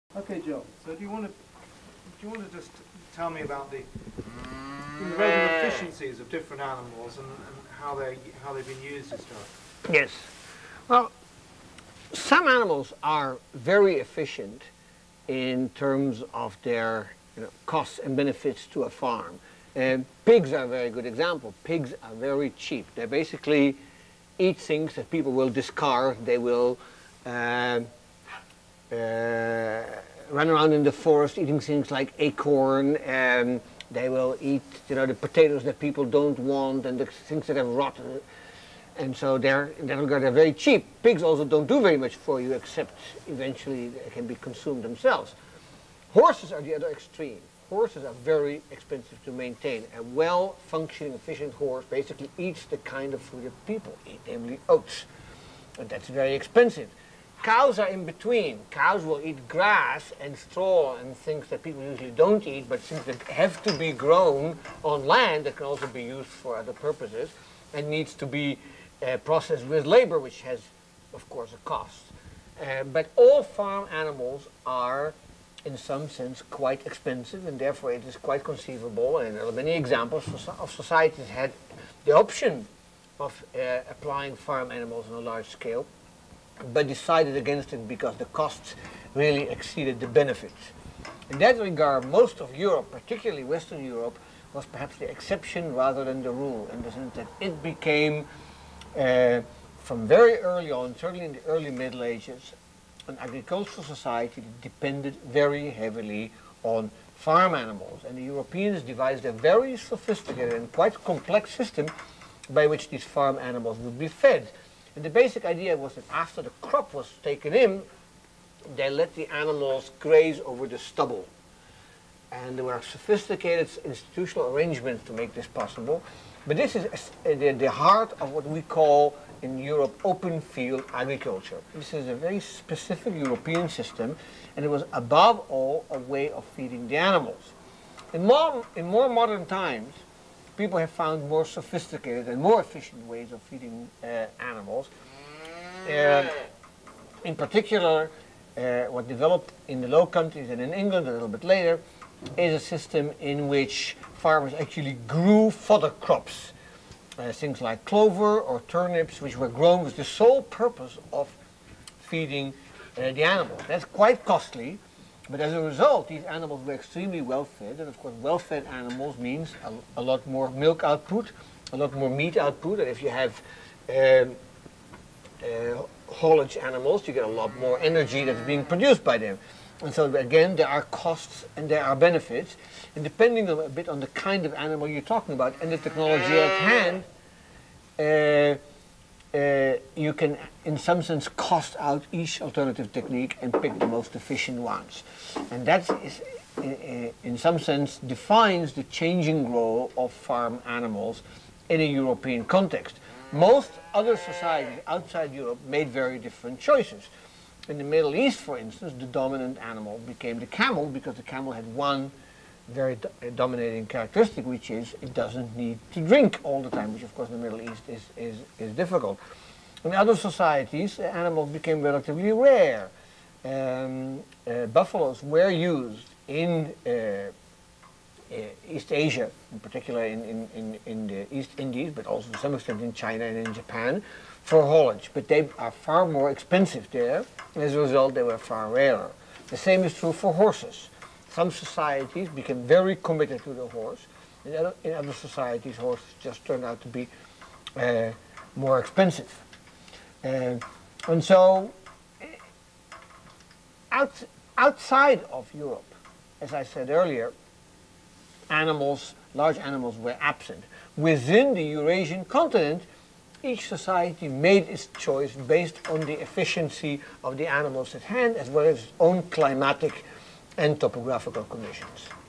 Joel Mokyr on the relative efficiency of different types of animal
Joel Mokyr explains the different efficiency of certain animals, pigs, horses, cows, the nature of open field agriculture, the growing of fodder crops, camels and buffaloes.